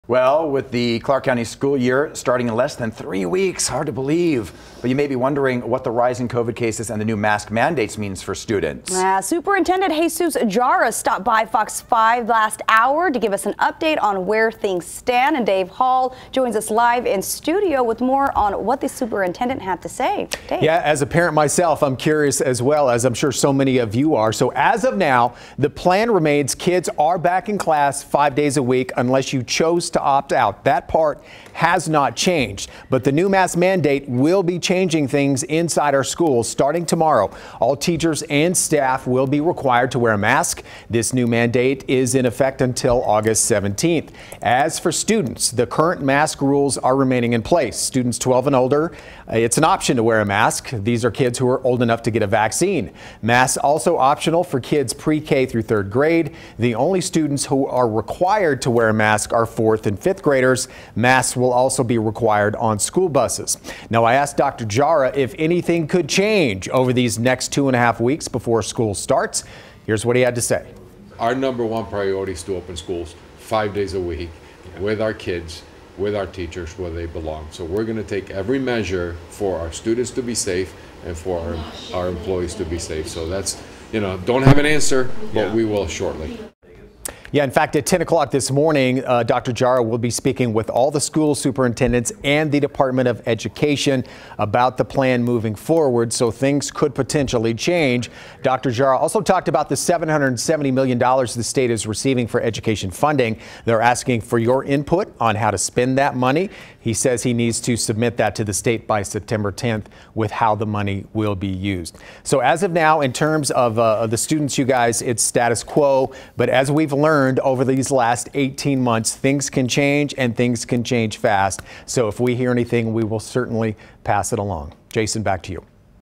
CCSD Supt. Jara discusses masks in schools
Given the ongoing spread of the Delta COVID-19 variant, Supt. Jesus Jara discussed masks in schools.